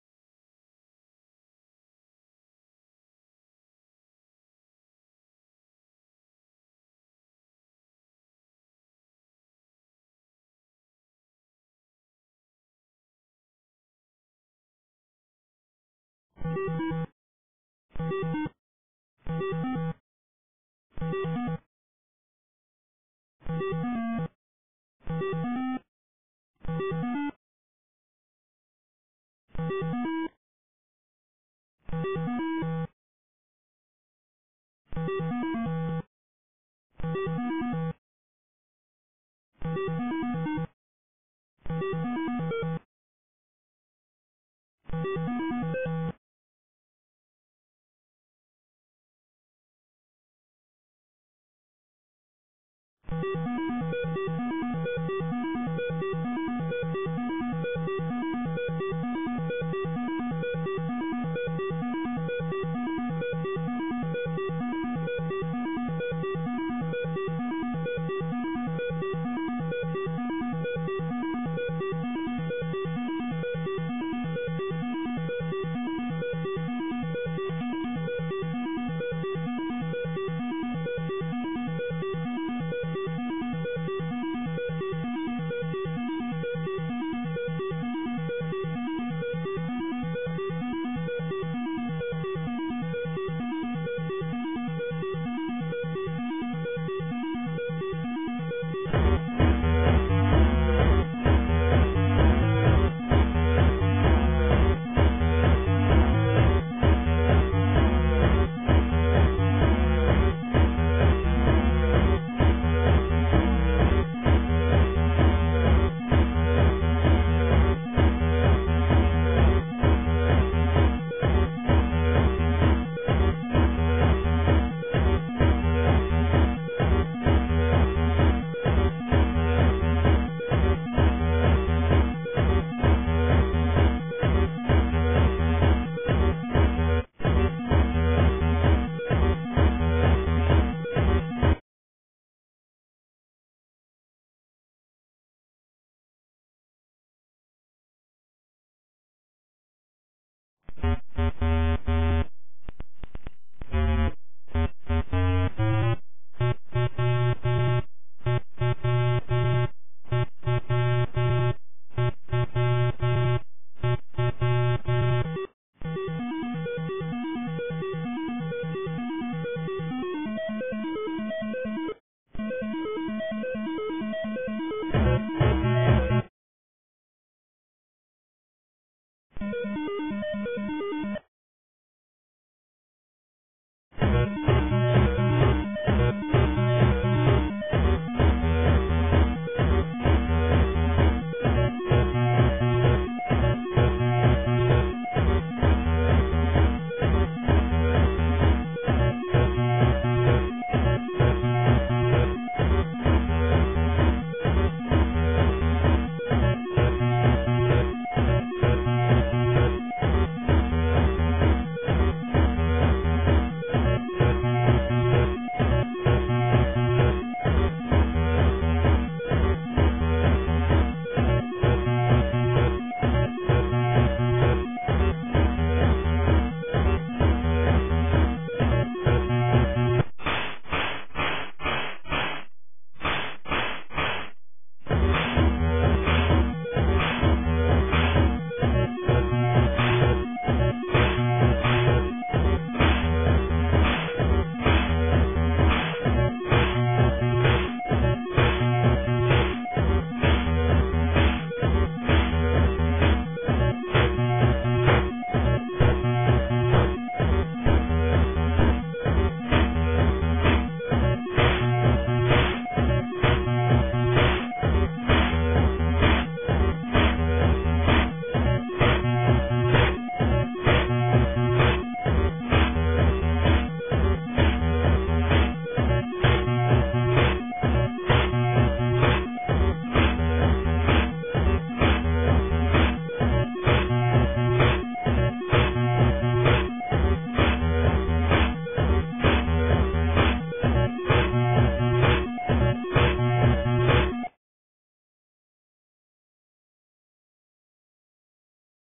Lsdj Tutorial Part Two (NOT QUITE A TUTORIAL)
PS: This video is old. And is actually not a tutorial,  just me doodling with LSDJ.